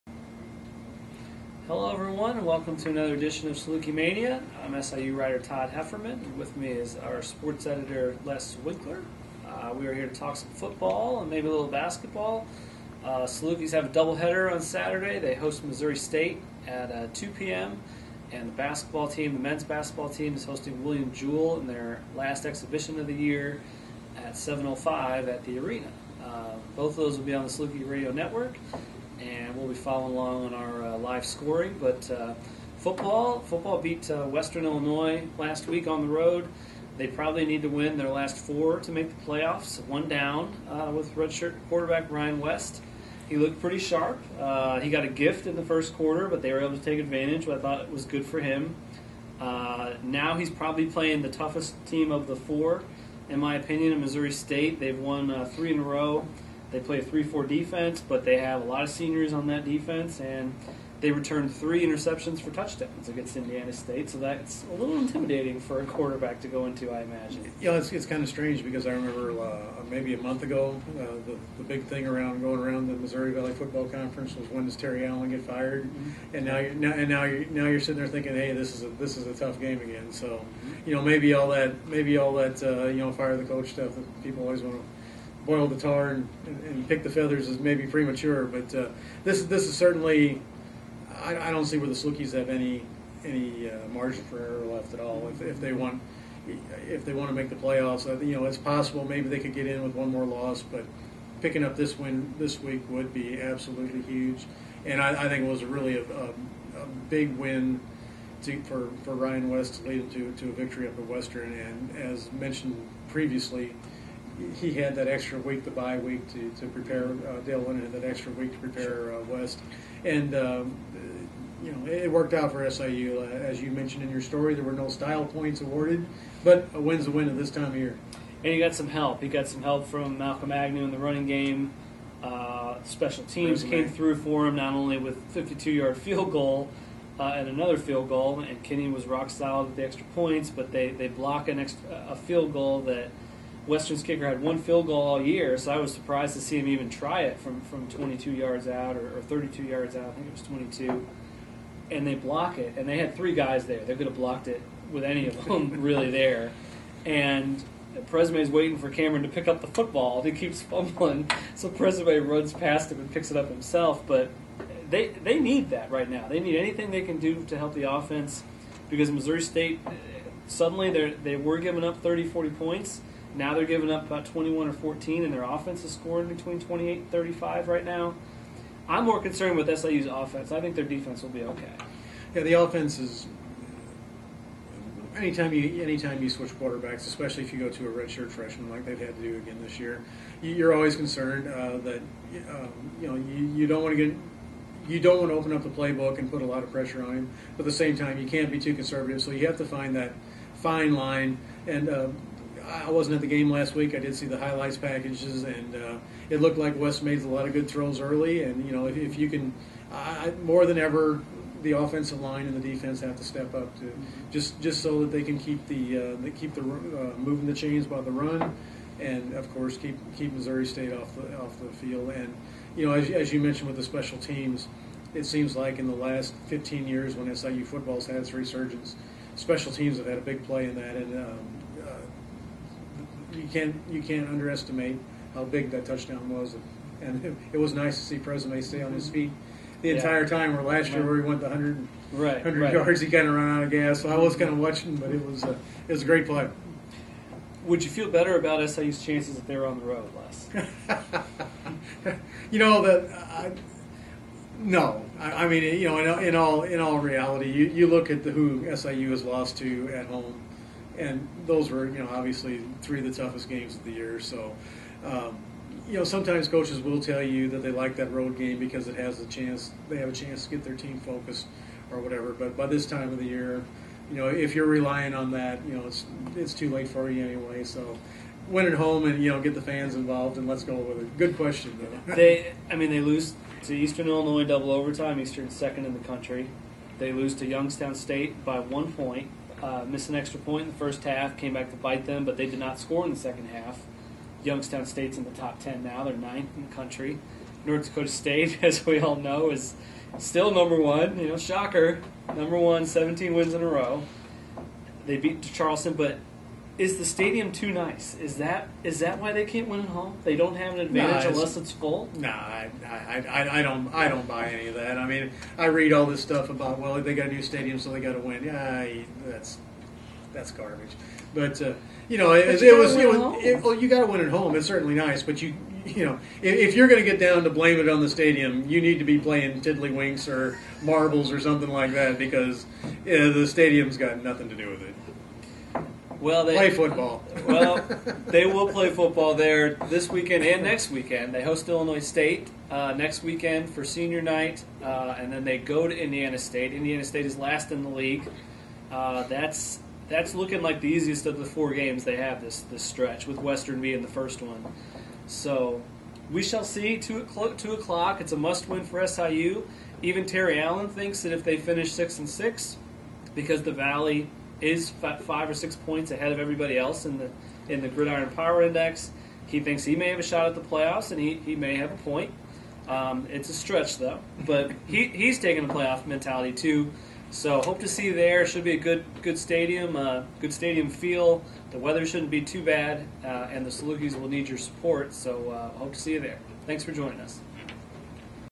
Salukimania for Nov. 7, 2013
Todd Hefferman and Les Winkeler discuss another week in Saluki football.